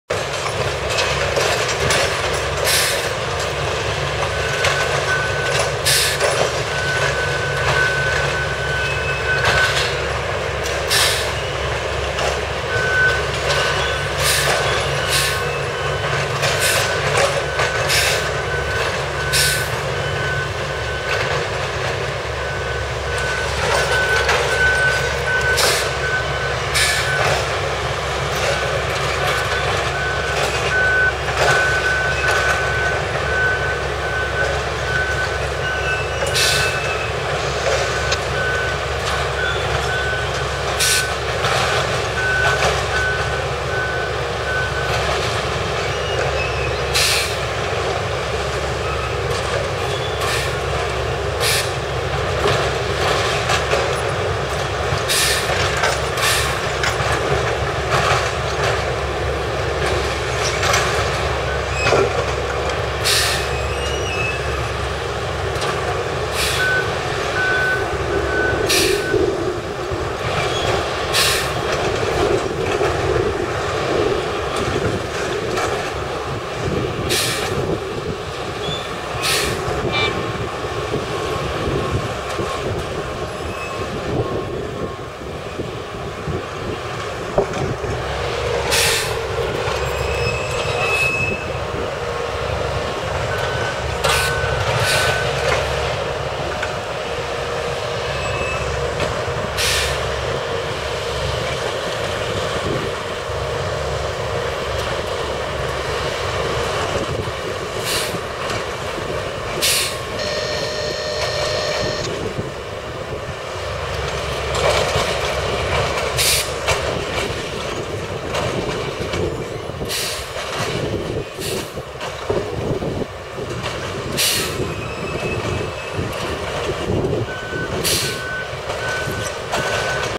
CSX Track Workers Replacing Railroad Ties
A CSX MOW vehicle replaces railroad ties near Montevideo Road.